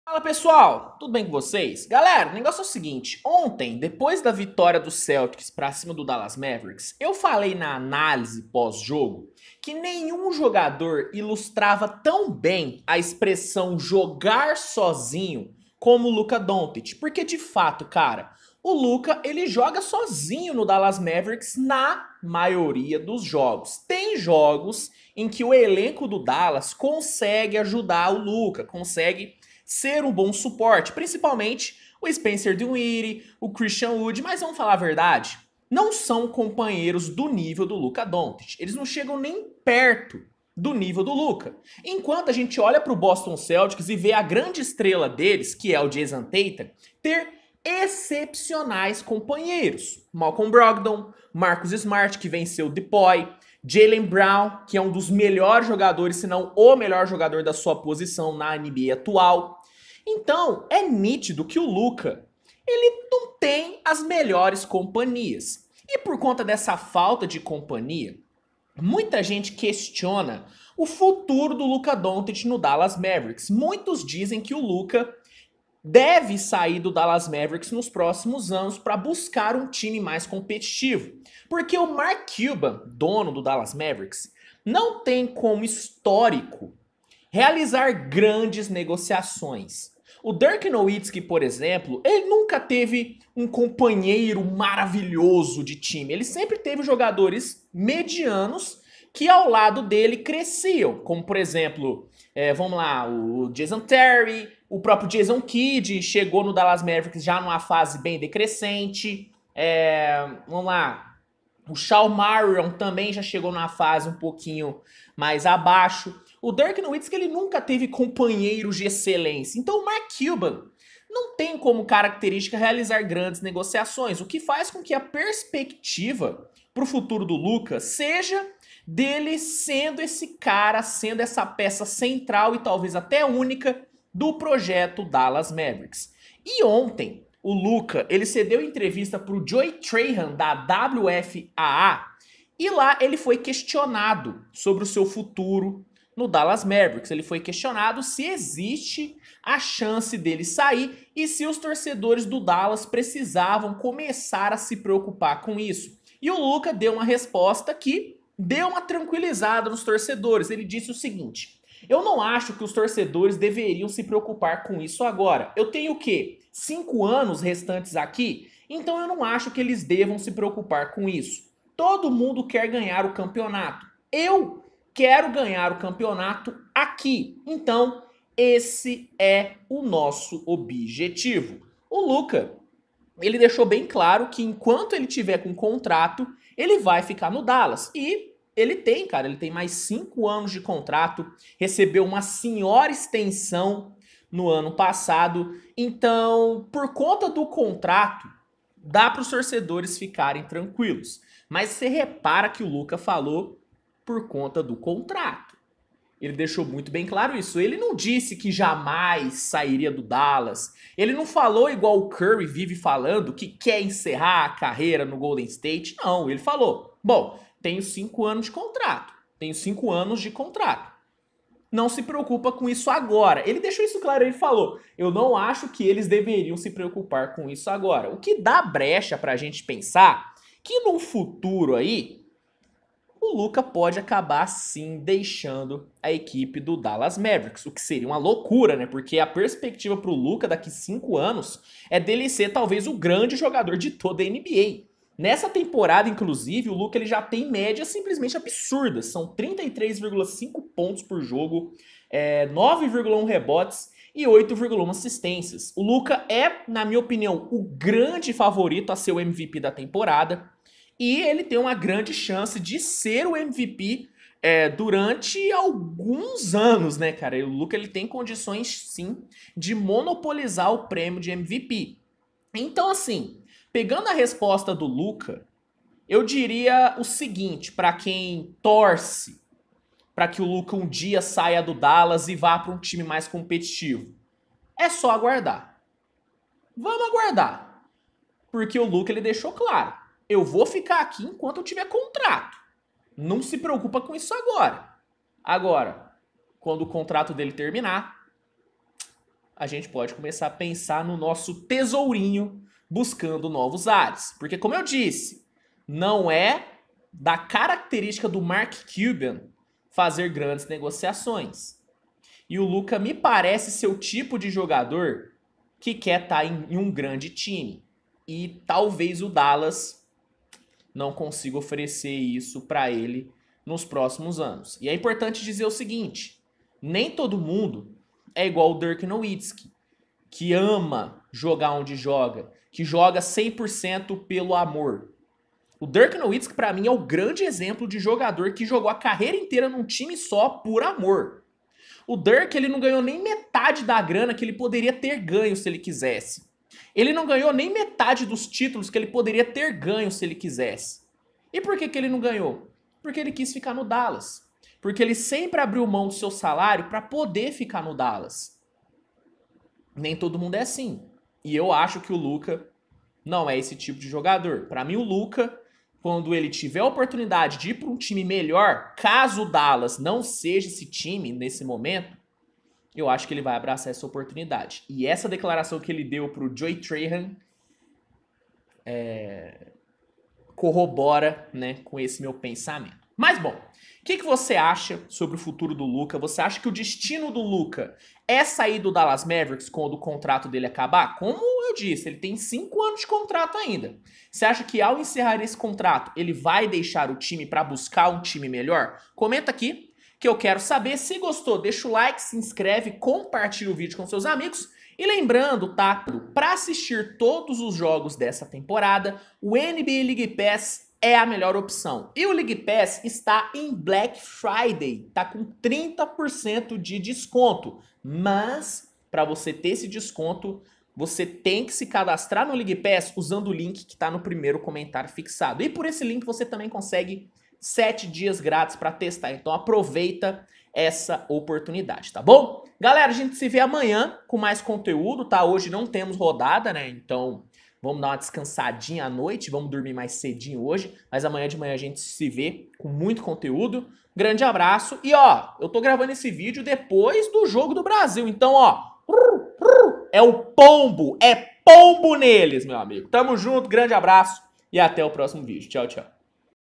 Luka Doncic FALA sobre FUTURO no Dallas Mavericks ( VAI SAIR ? )
Confira nesse vídeo a declaração de Luka Doncic sobre seu futuro no Dallas Mavericks !